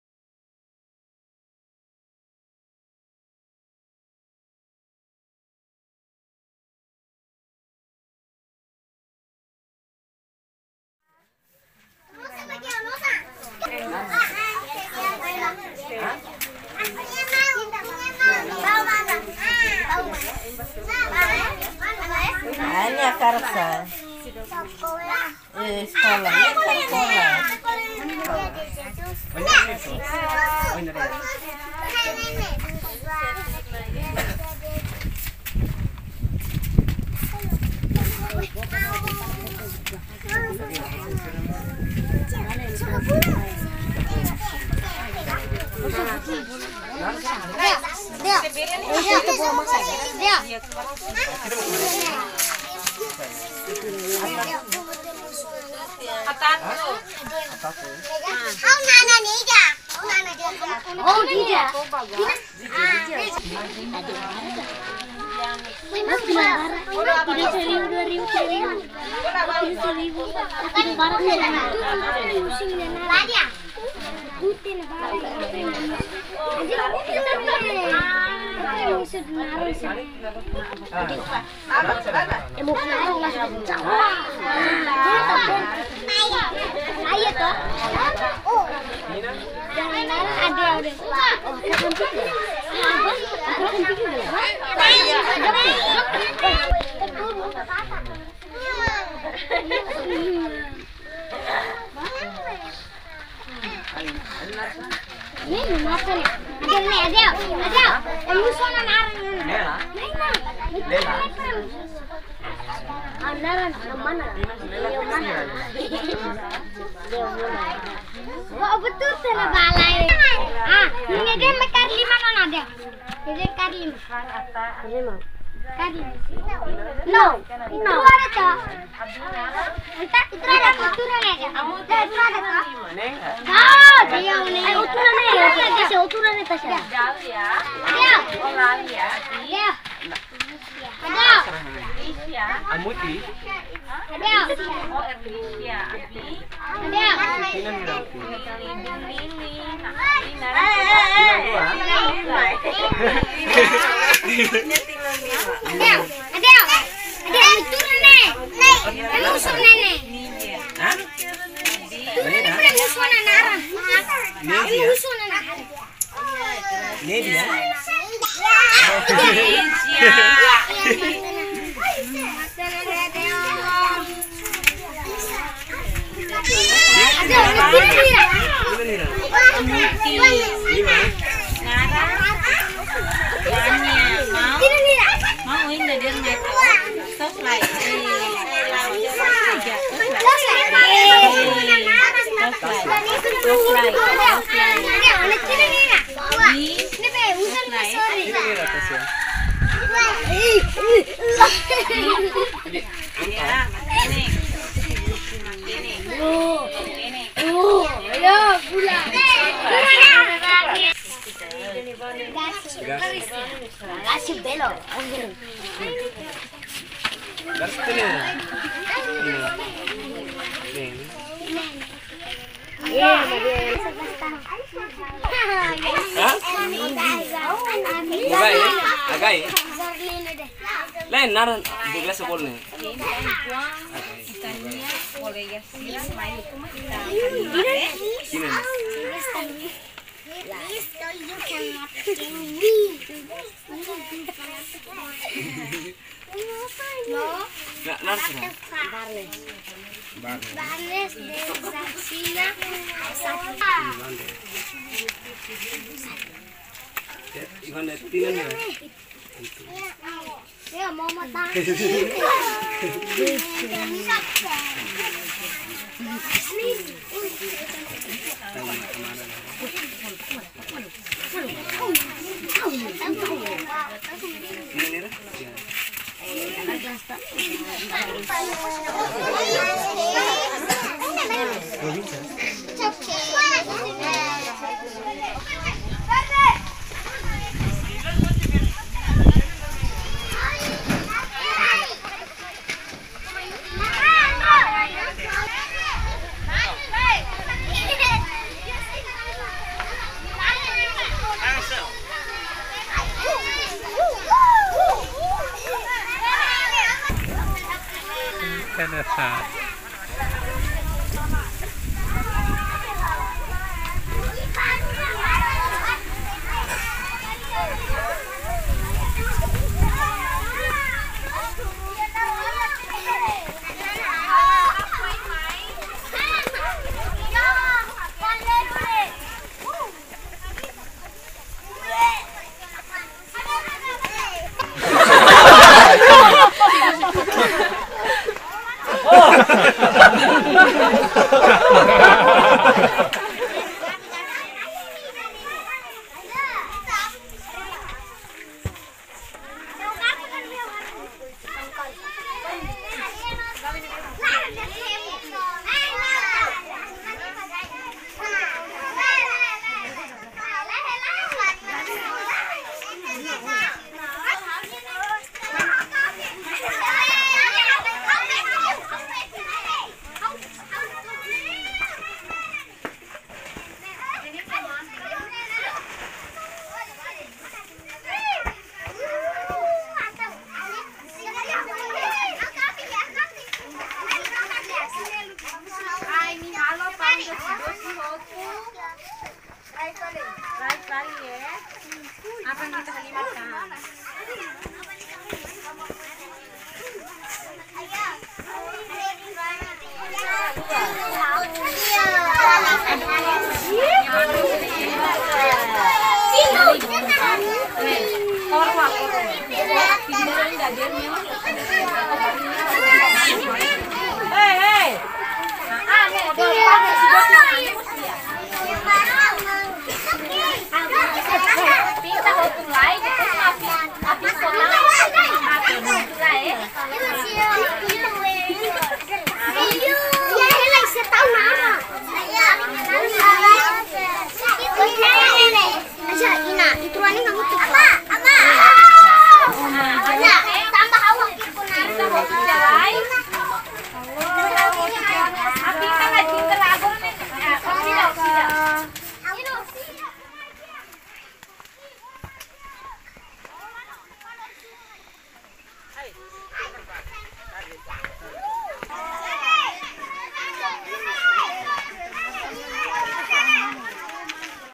Dede Dwi dan Kakak Putry ikut Program Unicef Part 2
Jadi ini hari Dede Dwi dan Kakak Putry bersama-temannya ikut Program Unicef di lingkungan St. Antonio C atau yang sering disebut Rai Kuak, 12 Oktober, Tasi-tolu Dili - Timor Leste.